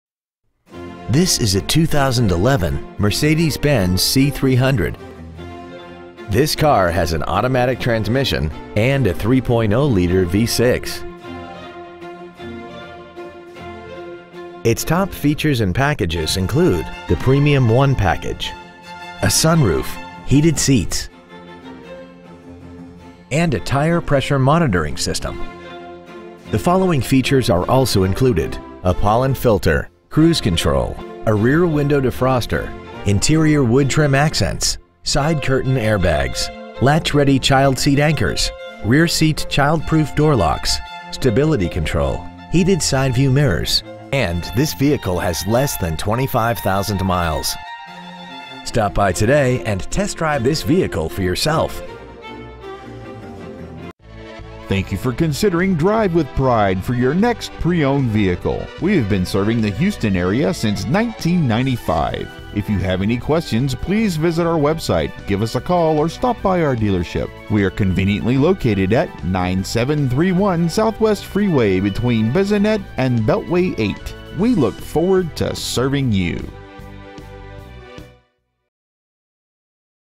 2011 Mercedes-Benz C300 Houston TX
We have been honored to serve the Houston TX area , we promise that your experience at our dealership will exceed your expectations ! Year : 2011 Make : Mercedes-Benz Model : C300 Engine : 3.0L DOHC 24-Valve V6 Engine Trans . : Automatic Exterior : Black Miles : 24,540 Interior : Black Stock : 388Z5294 Drive With Pride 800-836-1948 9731 Southwest Freeway Houston , TX 77074